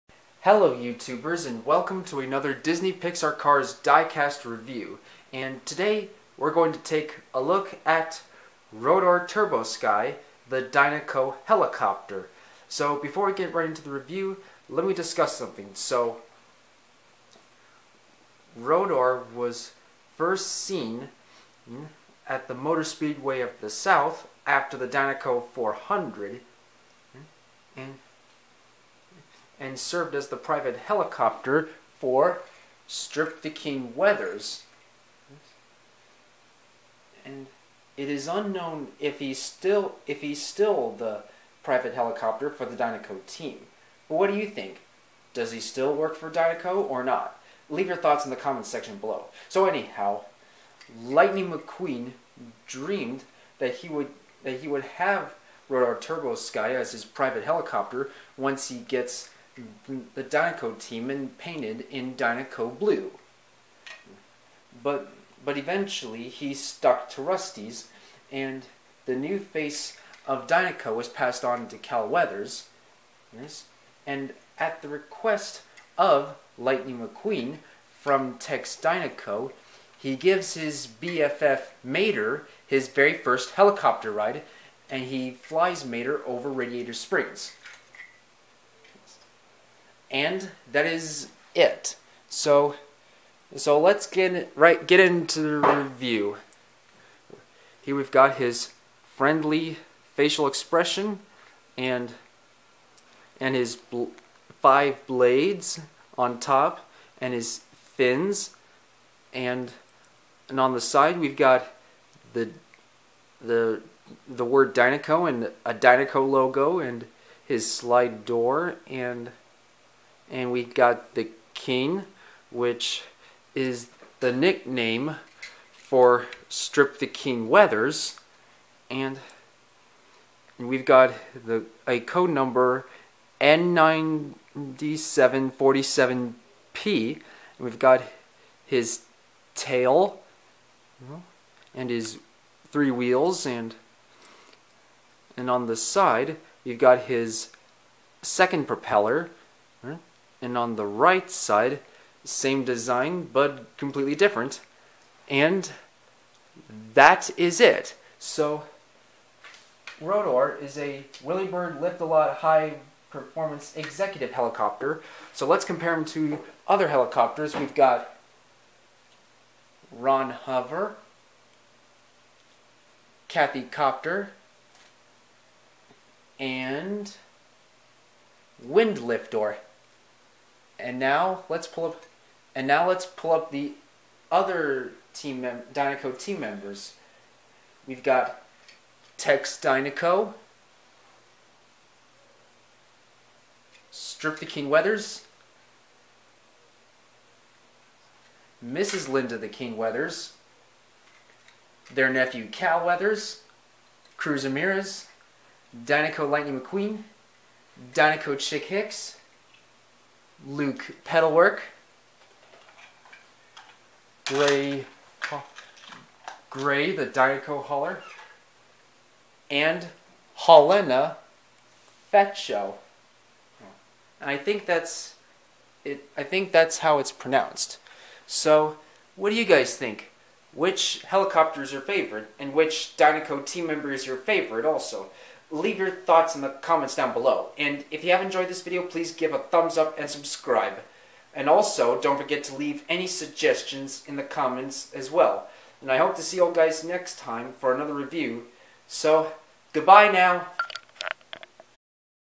Disney Pixar Cars Rotor Turbosky (Dinoco helicopter Piston Cup) diecast
Hello everybody & today, we're gonna take a look at Rotor Turbosky (Dinoco helicopter) diecast.
Cars belongs to Disney/Pixar.
All rihgts reserved!